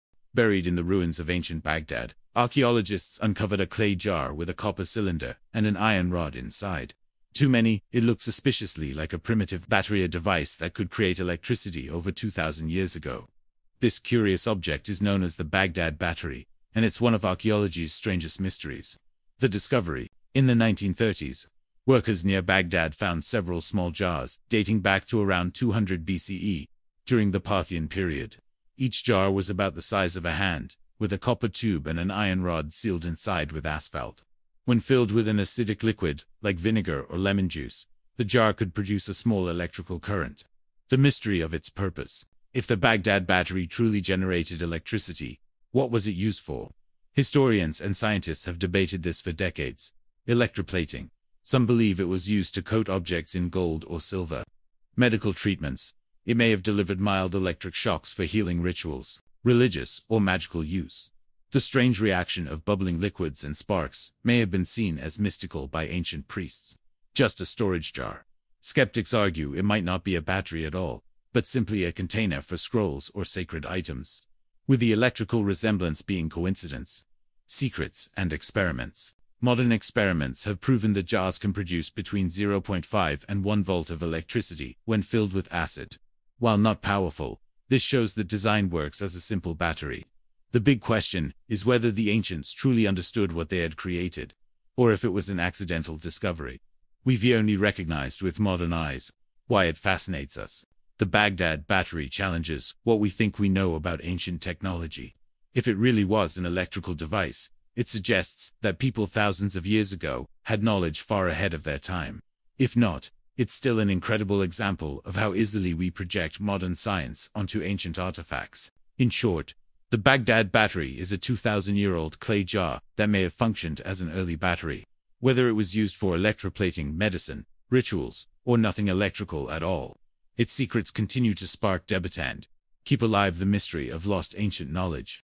Baghdad Battery – Ancient Electricity
Clay jars with copper — possibly primitive batteries; purpose unknown.